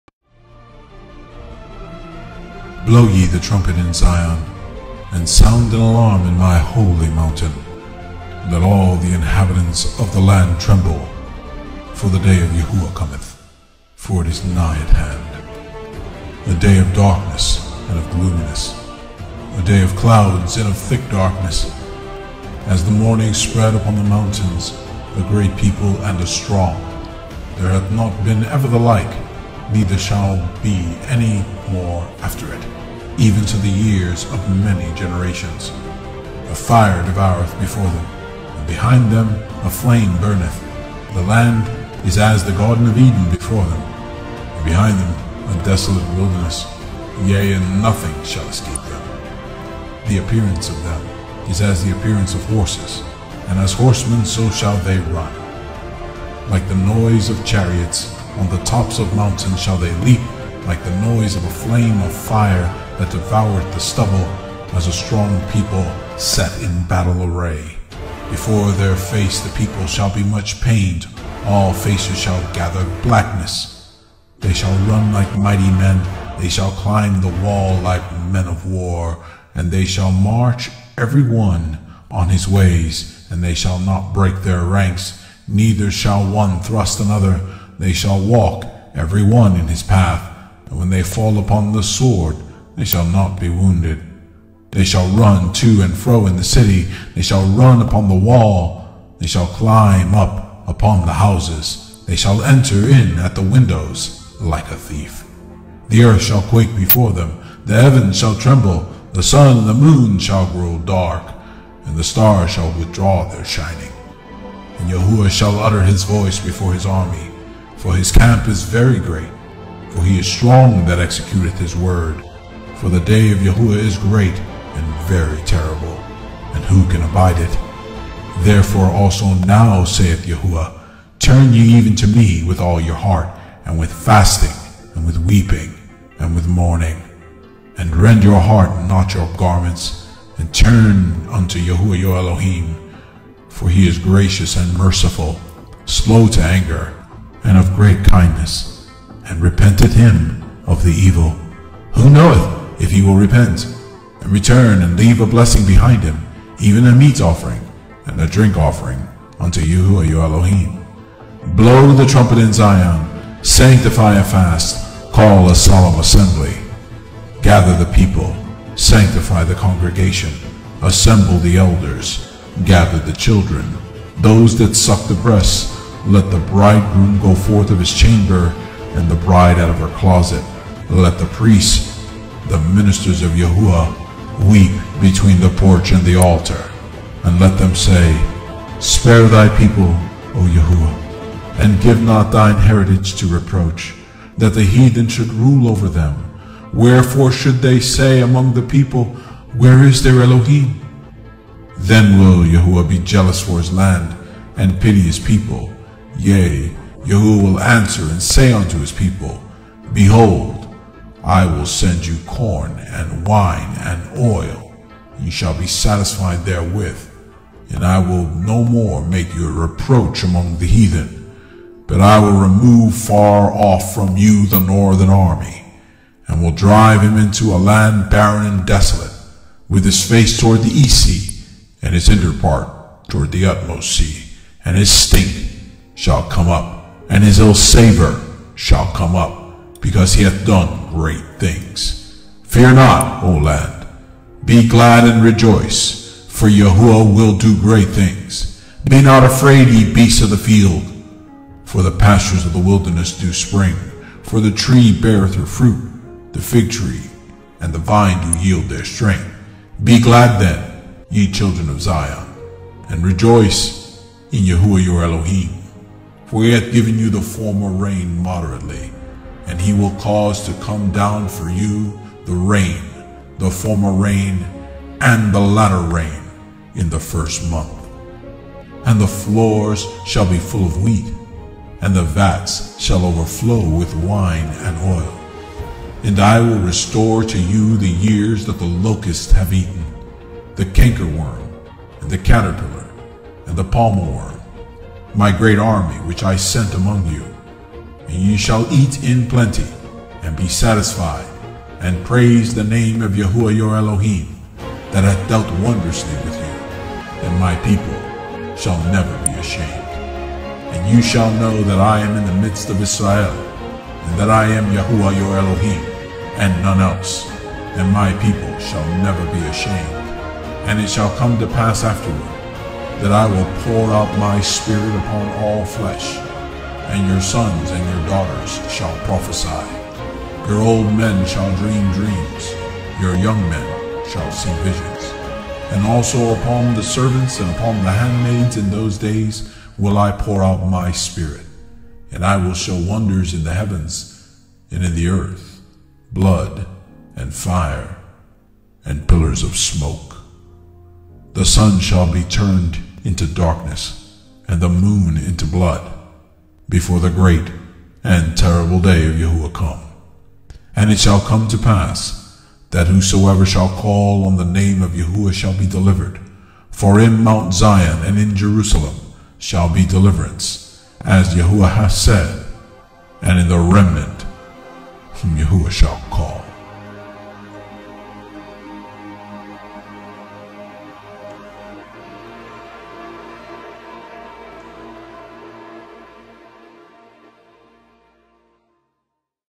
Joel 2: The Day Of Yahuah Cometh! - Remnant House
Joel chapter 2 as read by Peter Michael Martinez. A sober word for the days we are living in!